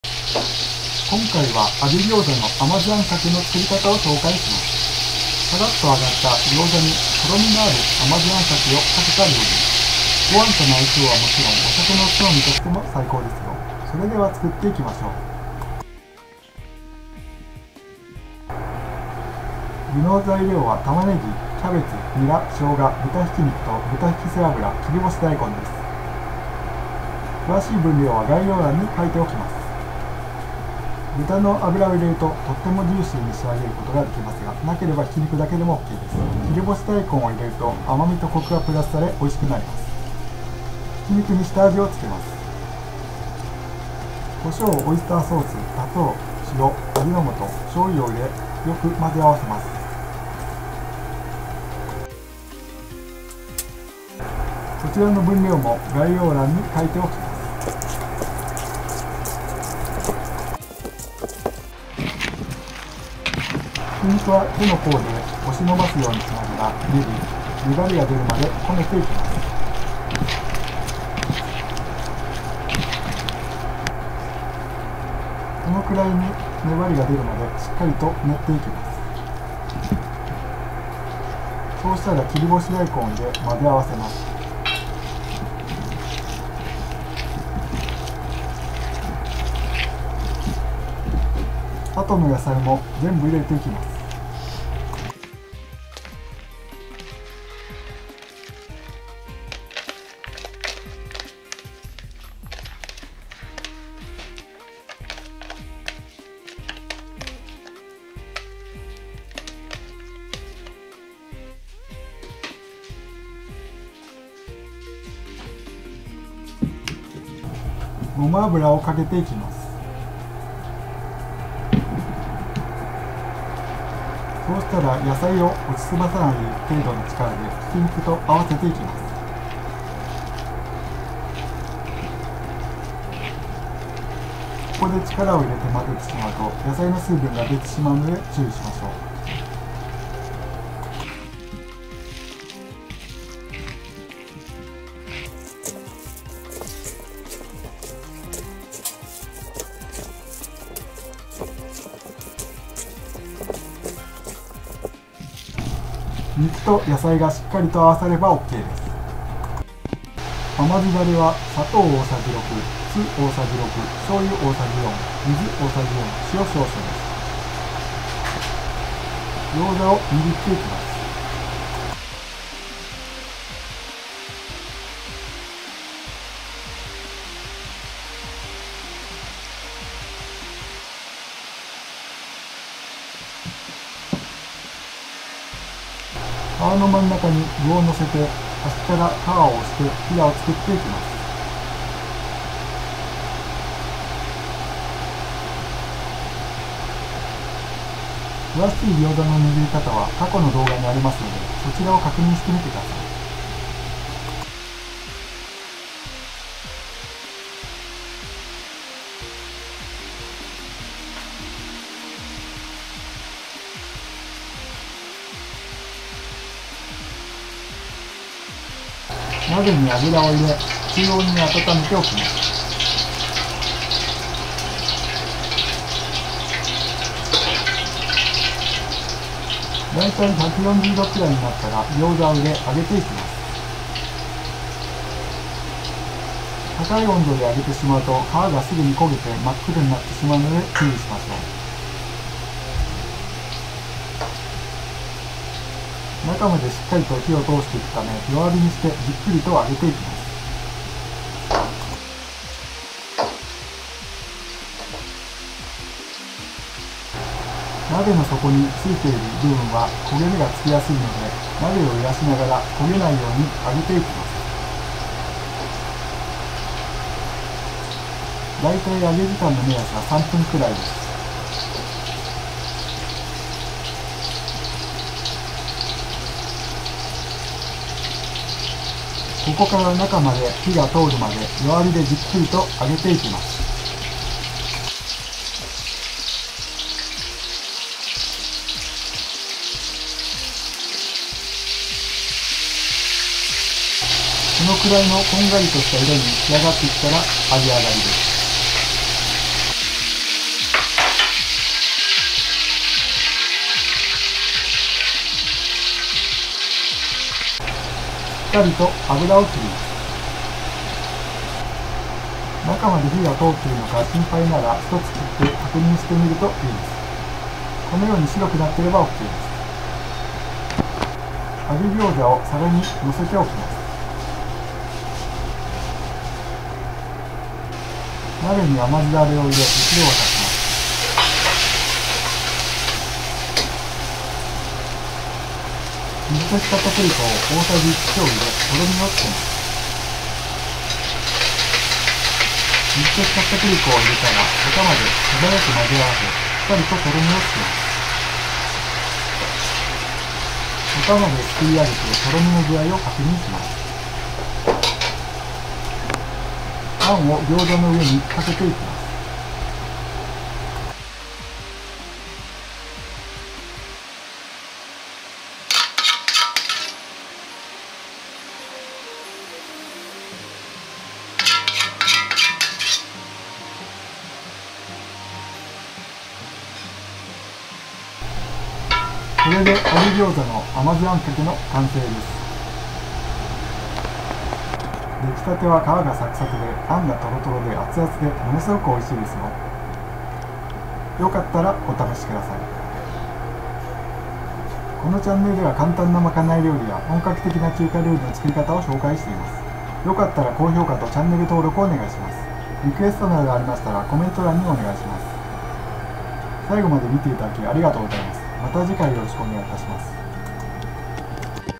プロが教える！揚げ餃子の甘酢あんかけの作り方
材料　豚ひき肉１５０g　豚背油５０g　キャベツ１００g、玉ねぎ1/4個
ニラ1/3束、生姜大さじ１　切り干し大根３０ｇ
下味　オイスターソース大１　醤油大２　砂糖大１　塩小１　味の素小１
胡椒適量　ごま油大１
甘酢ダレ　砂糖大６　酢大６　醤油大４　水大１０　塩少々
餃子の皮